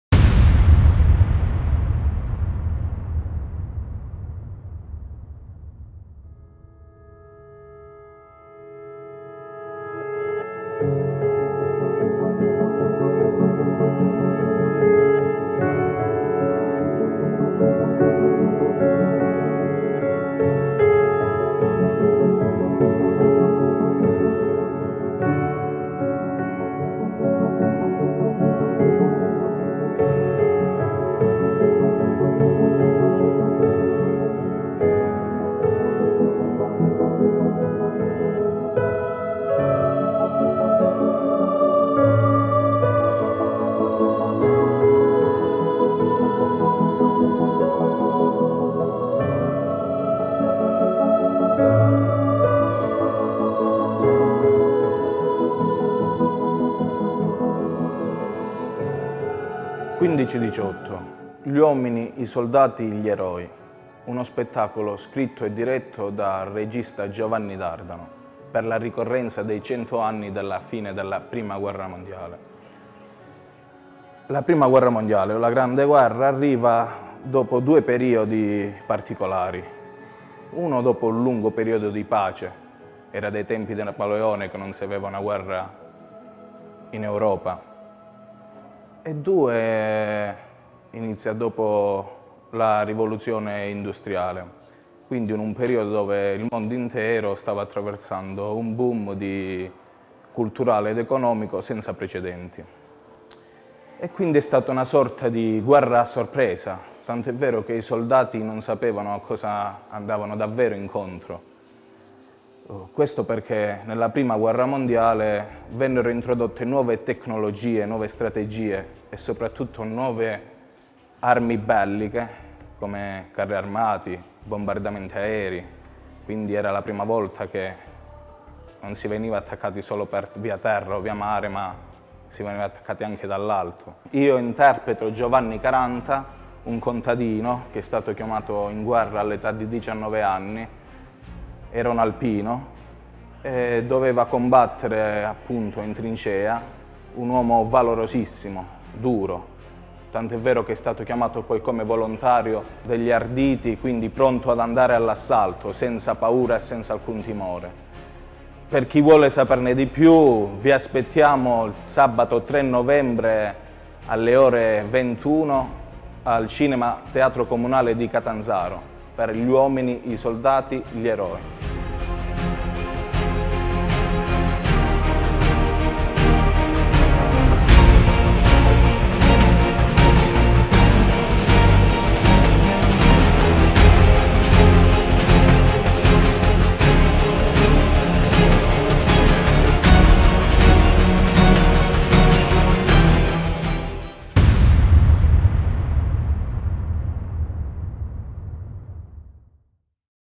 Vlog #2: L' Alpino - Giovanni Caranta
La grande guerra vista con gli occhi di chi l’ha vissuta: Bartolomeo Ristorto, Giovanni Caranta, Giovanni Battista Ponzo, Giovanni Tolosano, Maria Plozner Mentil, Maria Boni Brighenti, Erina Simoncelli, Giuseppe Daniele, Adamo Zanelli. Ripercorrendo l’arco temporale della prima guerra mondiale, lo spettacolo narra le storie di chi la Storia l’ha fatta veramente.